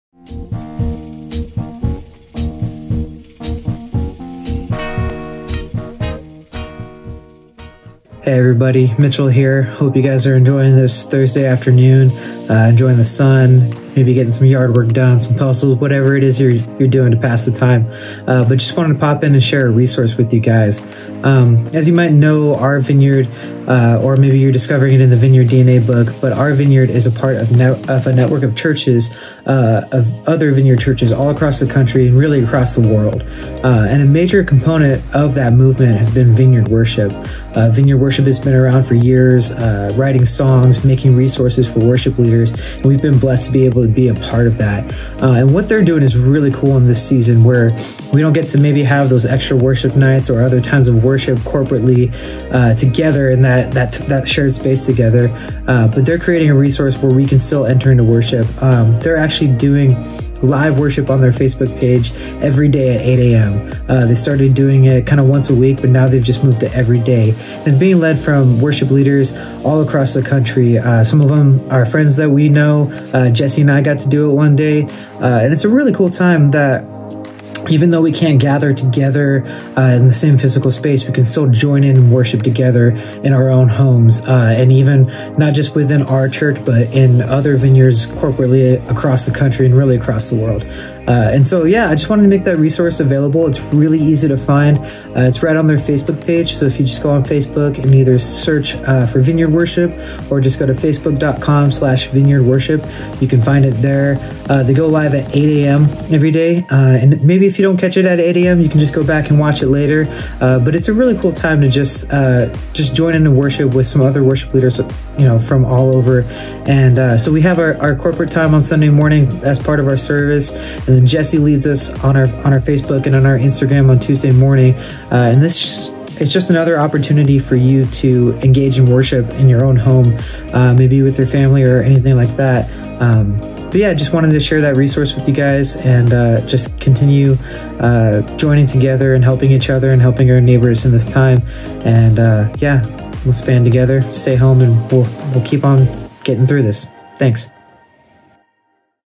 Heard it Through the Grapevine - April 16
Mitchell shares a resource from Vineyard Worship that we can use every day!